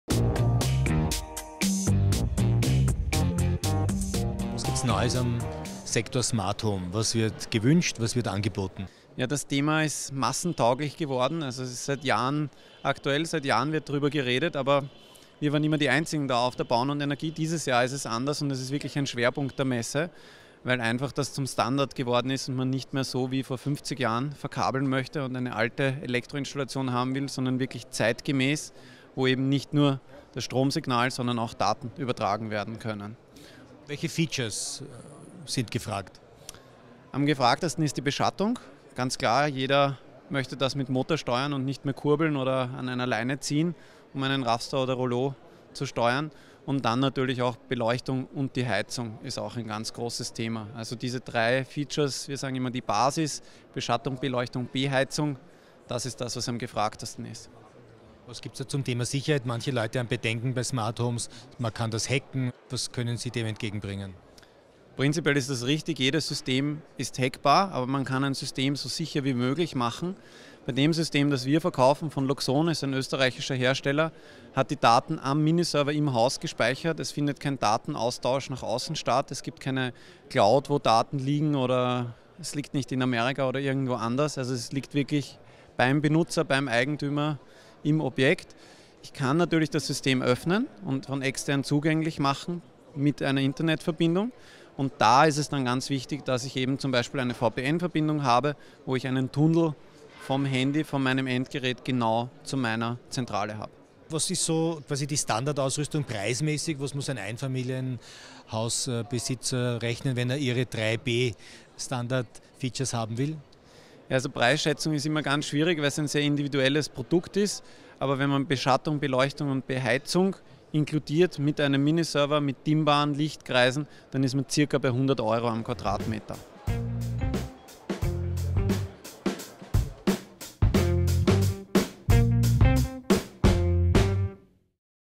smarthome360 auf der Messe Bauen & Energie 2018
Messerundgang auf der Bauen & Energie Wien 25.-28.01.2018. smarthome360 GF Mag. Stefan Mladek mit Infos und Trends zu smarthome Features. Produziert von